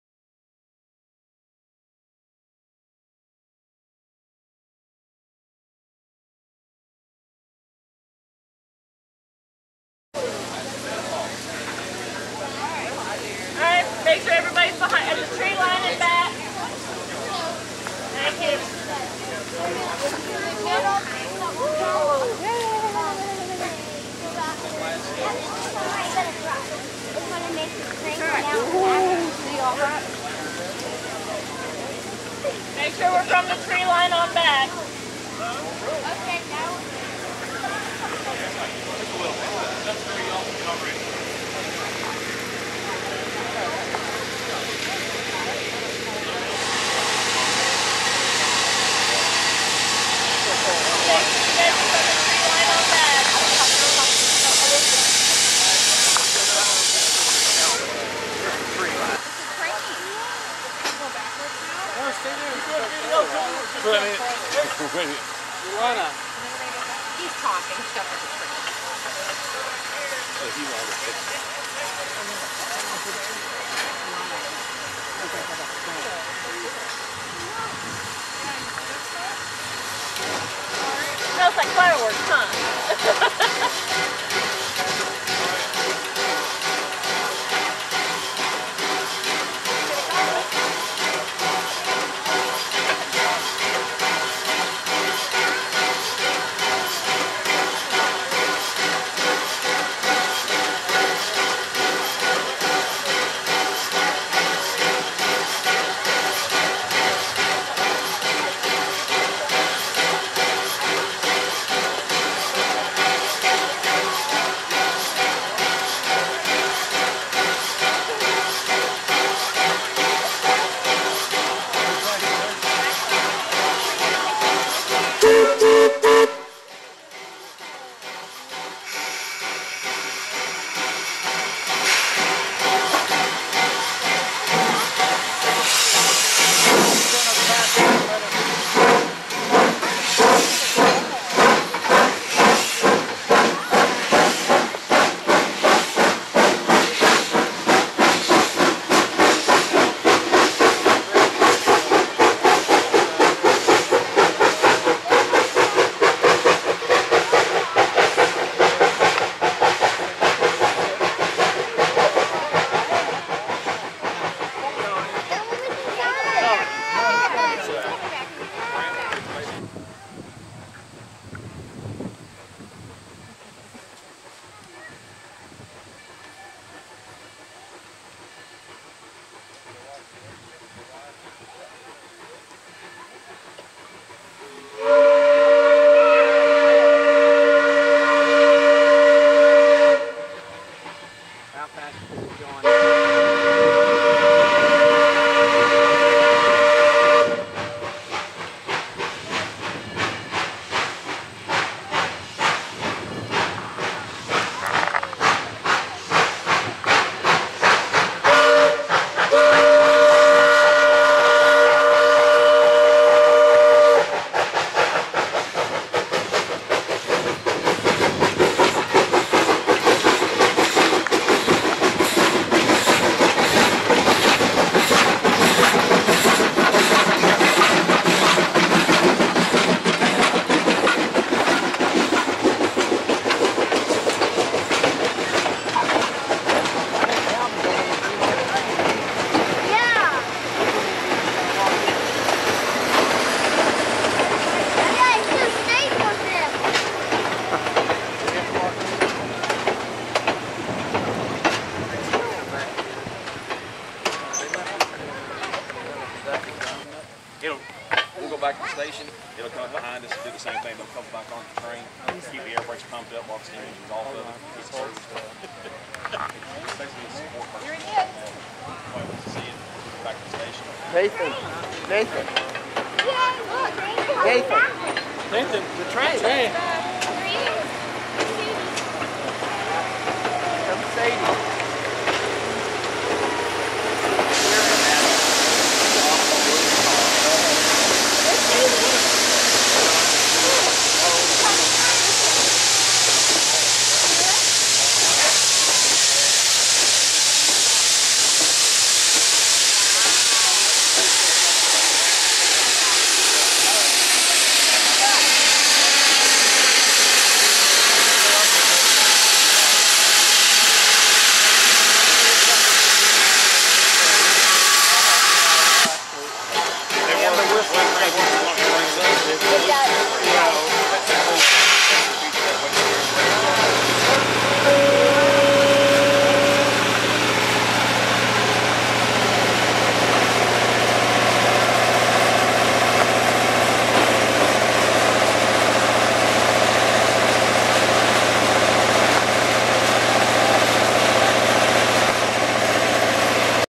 Bluegrass Railroad
The Bluegrass Railroad had a special coal engine in August 2016.